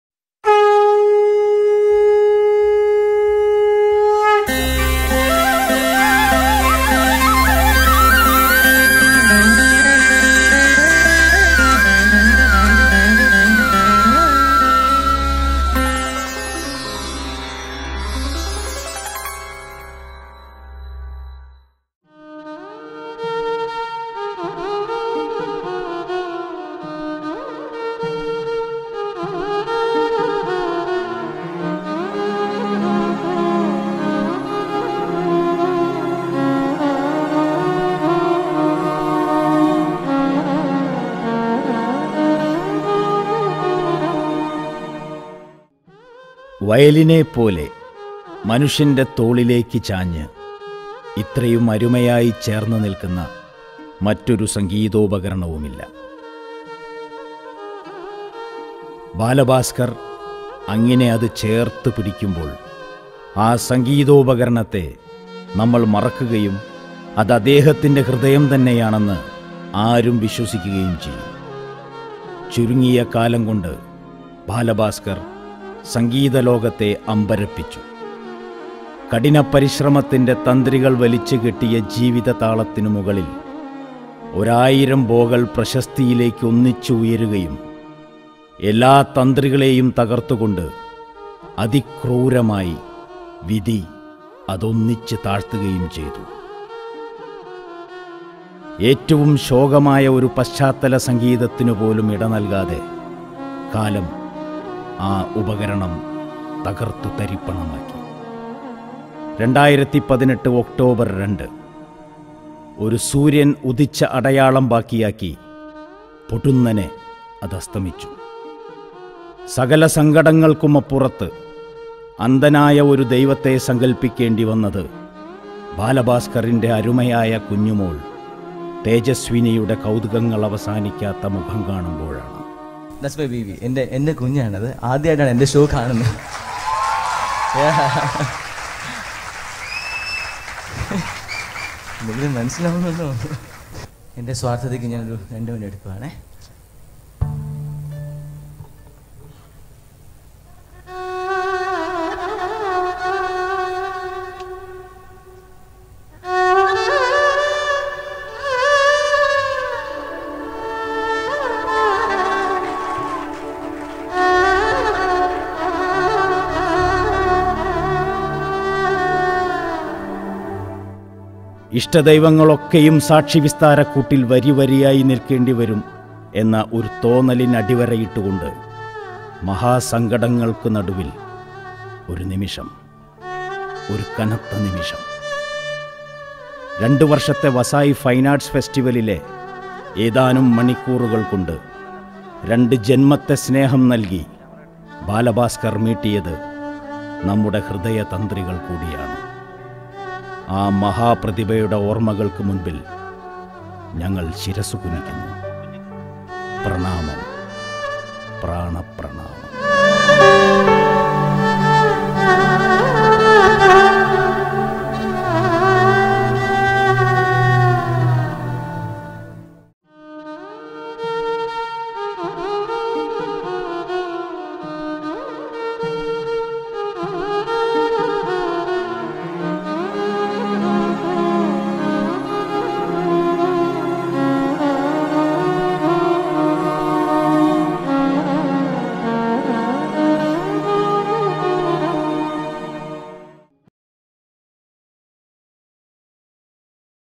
A TRIBUTE TO BALA BASKAR
കുറഞ്ഞ സമയം കൊണ്ട് വസായ് ജനതയുടെ മനം കവർന്ന ആ അനുഗ്രഹീതനായ ലോക പ്രശസ്‌ത വയലിൻ കലാകാരൻ ബാല ഭാസ്കറിന്റെ ഓർമ്മകൾക്ക് മുന്നിൽ വസായ് ഫൈൻ ആർട്സ് സൊസൈറ്റിയുടെ ആത്മപ്രണാമം

സ്ക്രിപ്റ്റ് - സ്വാതി മോഹൻ ഷൊർണുർ
ശബ്ദം - പ്രസാദ് ഷൊർണൂർ
എഡിറ്റിങ് - ജിബിൻ മീഡിയ ട്രെൻഡ്‌സ് തിരുവനന്തപുരം
ക്യാമറ - മീഡിയ ട്രെൻഡ്‌സ് തിരുവനന്തപൂരം
സ്റ്റുഡിയോ - സ്വർസ്വർഗ്ഗ് മുളുണ്ട് മുംബൈ
വേദി - വസായ് ഫൈൻ ആർട്സ് ഫെസ്റ്റിവൽ 2019